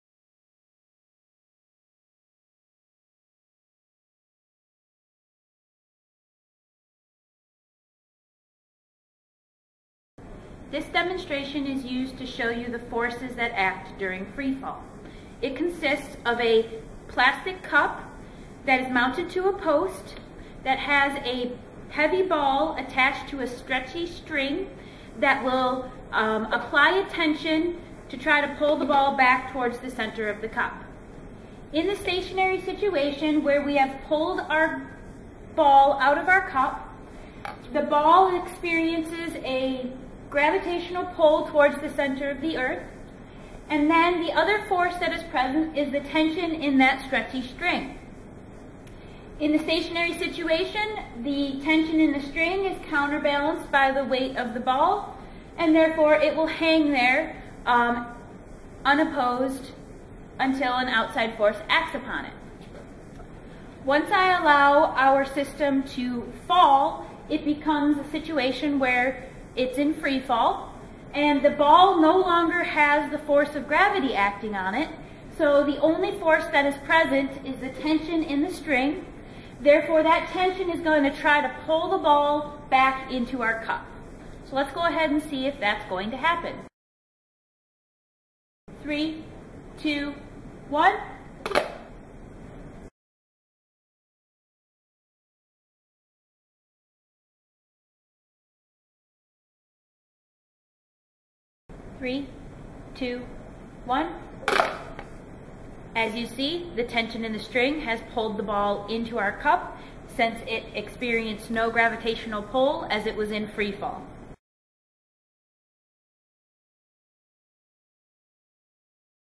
1G20.38 - Forces acting during Free fall
To demonstrate zero gravity during free fall (weightlessness)